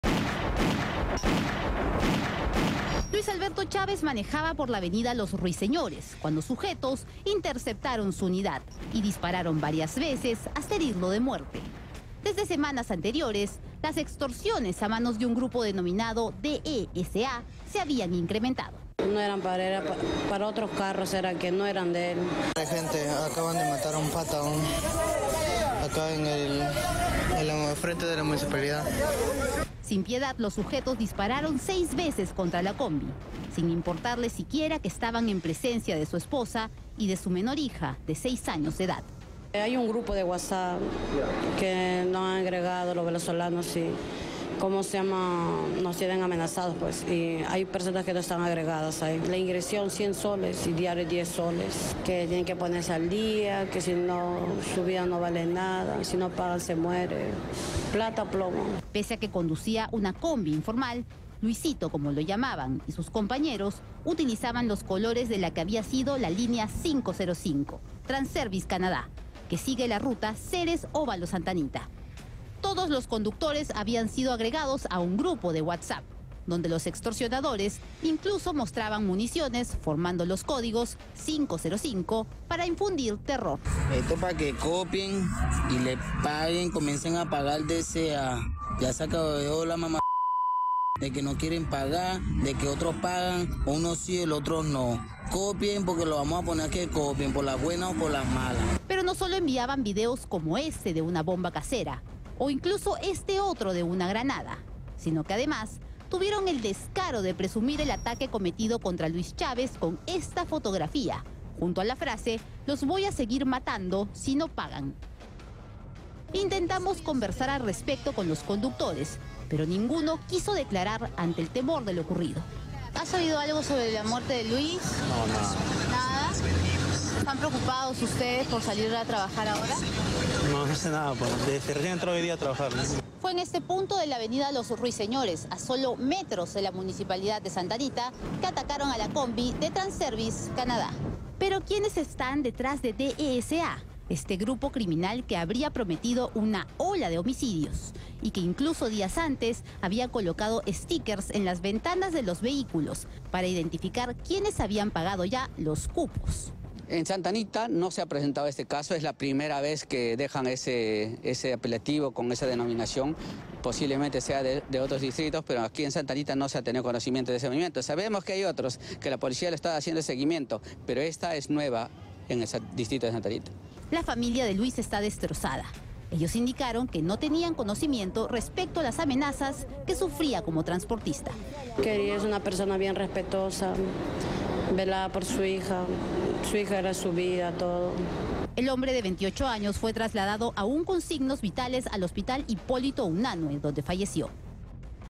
Santa Anita: banda criminal 'D.E.S.A.' estaría detrás de ataque a chofer
Luis Alberto Chávez, un conductor de 28 años, fue víctima de un brutal ataque mientras conducía su combi de la exlínea 505 por la avenida Los Ruiseñores, en Santa Anita. Sujetos armados interceptaron su unidad y le dispararon frente a su esposa y su hija de seis años.

Semanas antes, Chávez y otros conductores habían recibido amenazas de un grupo criminal conocido como D.E.S.A., que extorsionaba a transportistas informales de la ruta Ceres-Óvalo Santa Anita.

Según testimonios de familiares, los extorsionadores exigían un pago inicial de 100 soles y una cuota diaria de 10 soles, bajo amenazas de muerte. "Plata o plomo", decían en mensajes enviados a un grupo de WhatsApp, donde incluso compartían videos de granadas y bombas molotov.

DELINCUENTES PRESUMIERON EL ATAQUE

Chávez fue trasladado al Hospital Hipólito Unanue, donde lamentablemente falleció debido a la gravedad de las heridas. En un acto de cinismo, los criminales presumieron el ataque compartiendo una fotografía del vehículo baleado junto al mensaje: "Los voy a seguir matando si no me pagan".